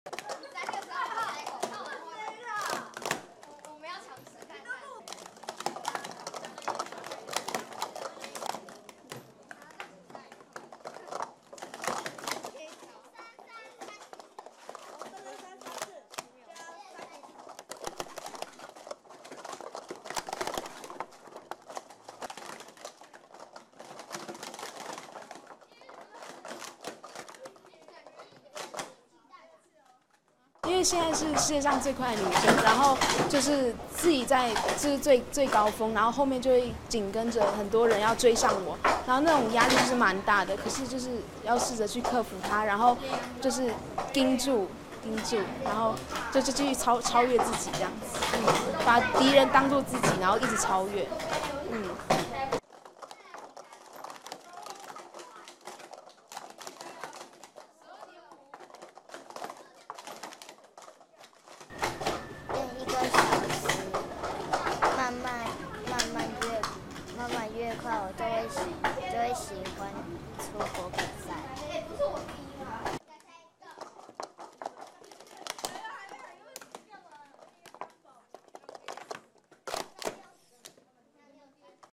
Taiwanese team stack up gold medals BBC News
The speed stacking team from Taiwan have returned to training after winning 11 gold medals at the world championships in Germany.